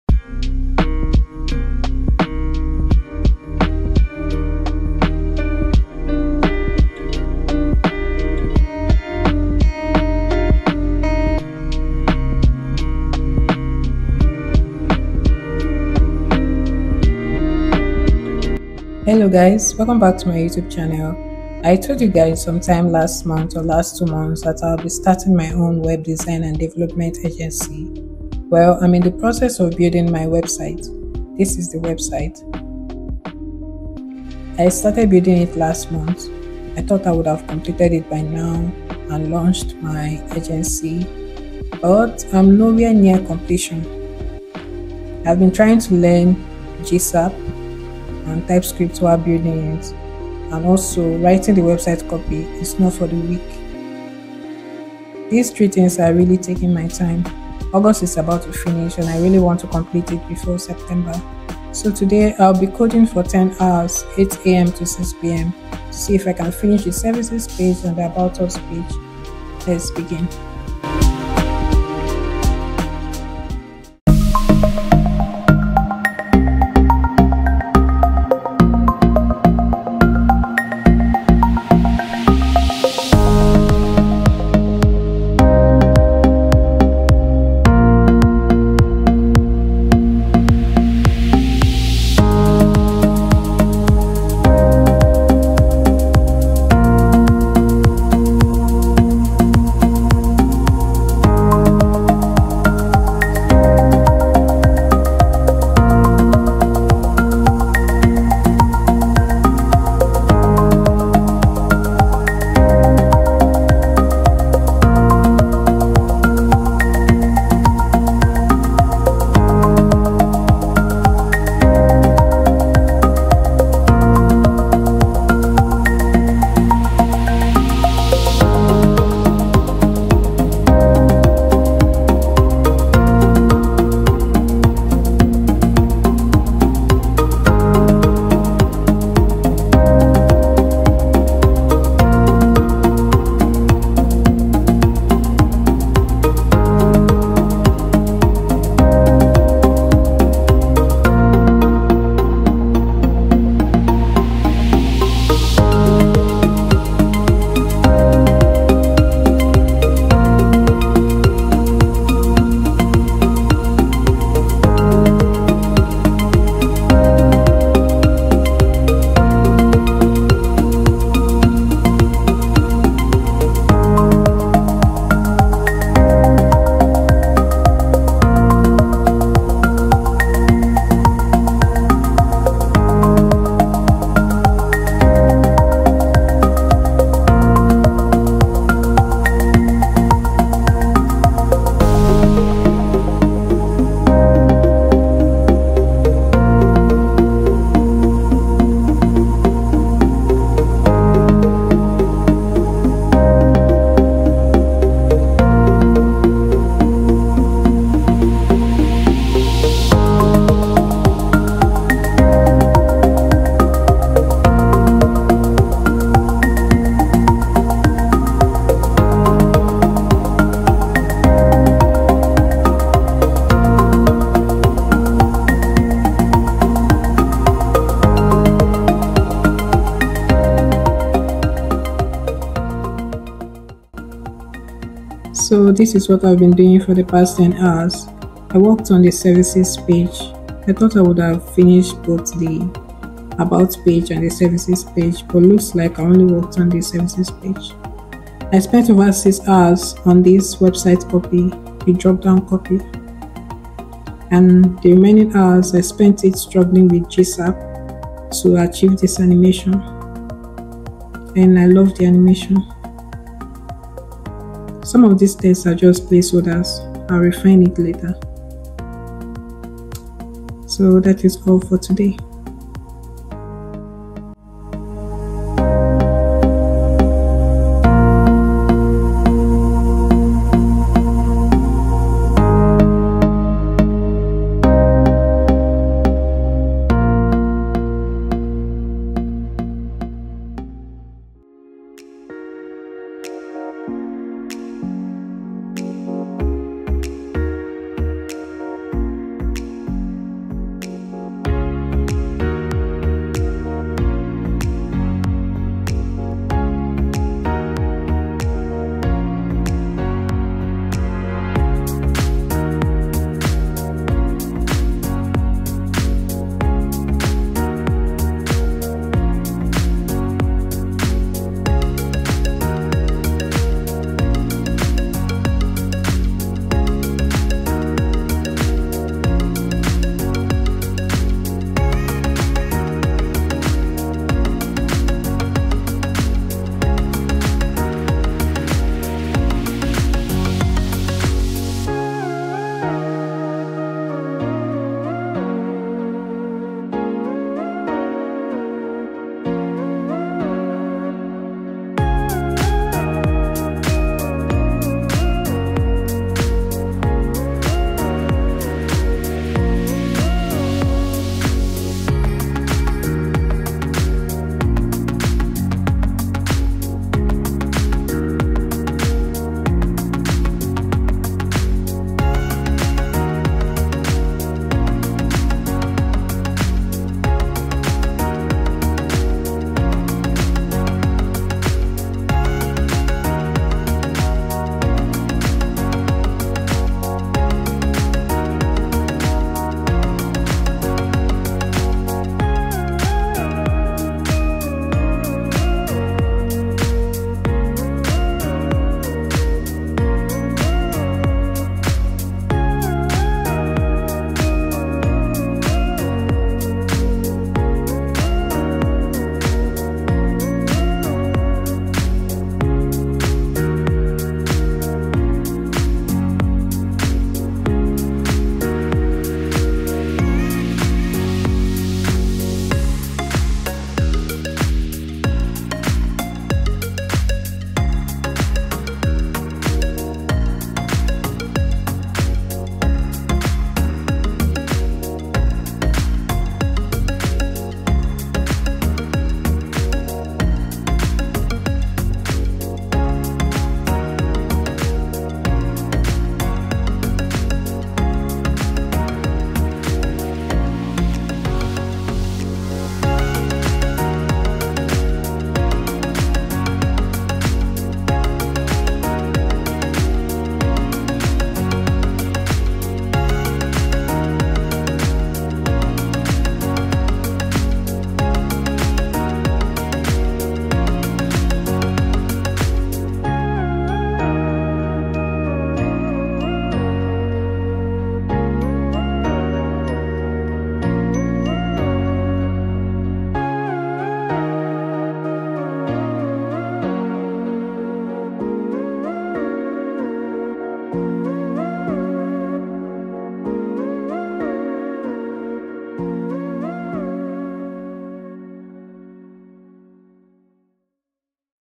A Coding Vlog: Building My Web Design and Development Agency Website
Building EMILIQUÉ STUDIO

💖 Timestamps 💖
0:00 - Intro
1:14 - Coding Vlog
3:54 - End Result

🌟 Thank You, Lovelies! 🌟